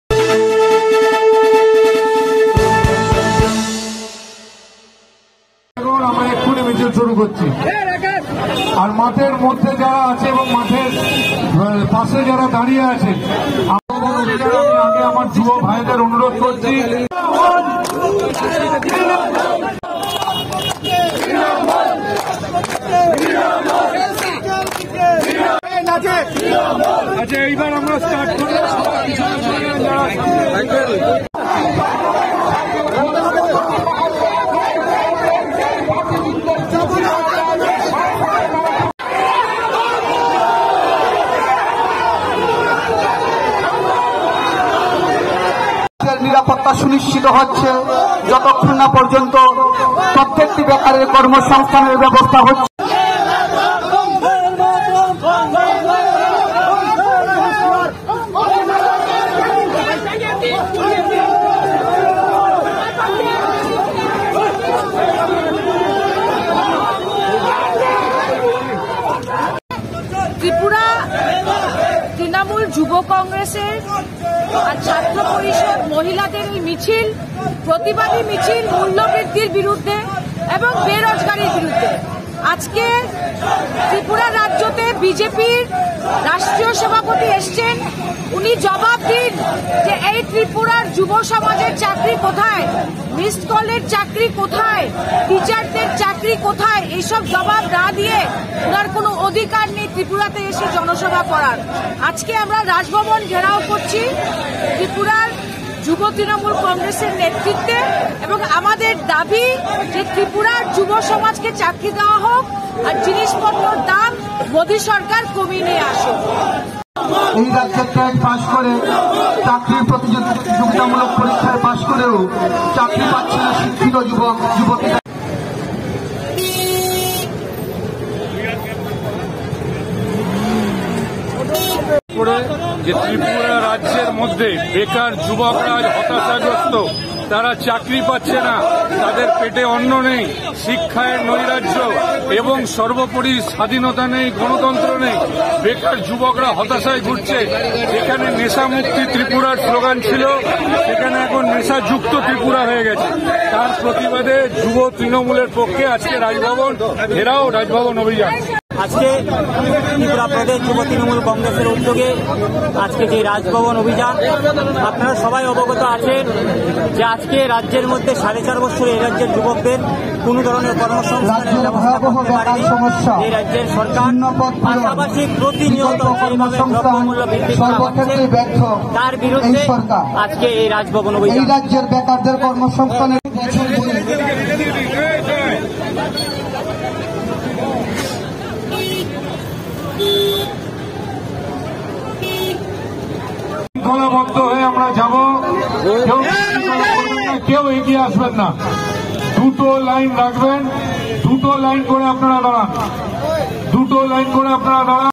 Triprua TMC : দ্রব্যমূল্য বৃদ্ধি সহ একাধিক দাবিতে যুব তৃণমূল কংগ্রেসের রাজভবন অভিযান।
দ্রব্যমূল্য বৃদ্ধি সহ একাধিক দাবিতে যুব তৃণমূল কংগ্রেসের রাজভবন অভিযান।